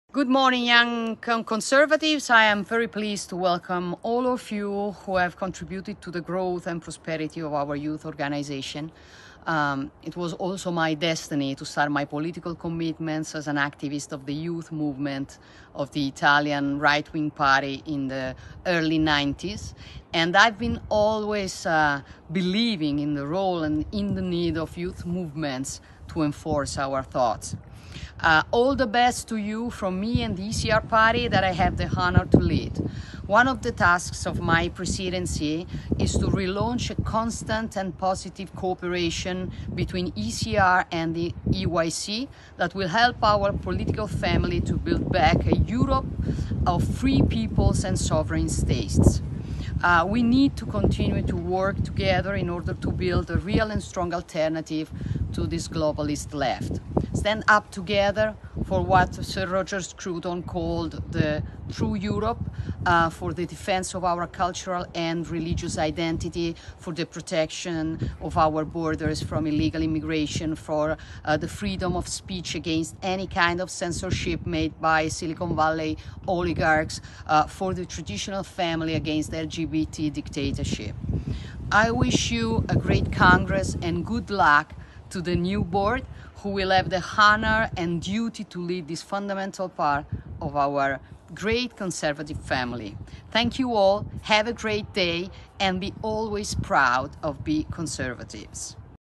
Meloni a EYC: Conservatori al lavoro per alternativa a sinistra globalista e difendere identità
«Dobbiamo continuare a lavorare insieme per costruire un'alternativa reale e forte a questa sinistra globalista. Batterci insieme per quella che Sir Roger Scruton ha definito la nostra “vera Europa”, per la difesa della nostra identità culturale e religiosa, per la protezione dei nostri confini dall'immigrazione illegale, per la libertà di parola contro ogni tipo di censura fatta dagli oligarchi della Silicon Valley, per la famiglia naturale contro la dittatura LGBT».

Lo ha detto il presidente di Fratelli d’Italia e di ECRParty, Giorgia Meloni, in un video messaggio inviato al congresso di EuropeanYoungConservatives (EYC), l’organizzazione giovanile dei conservatori europei.

«Uno dei compiti della mia presidenza - ha aggiunto Meloni - è rilanciare una cooperazione costante e positiva tra ECR e EYC, che aiuterà la nostra famiglia politica a ricostruire un'Europa di popoli liberi e nazioni sovrane. È stato anche il mio destino iniziare i miei impegni politici come attivista del movimentogiovanile della destra italiana, all'inizio degli anni '90. E ho sempre creduto nel ruolo e nella necessità dei movimenti giovanili per rafforzare il nostro pensiero. I miei migliori auguri da parte mia e di ECR, il partito che ho l'onore di guidare. Siate sempre orgogliosi di essere conservatori».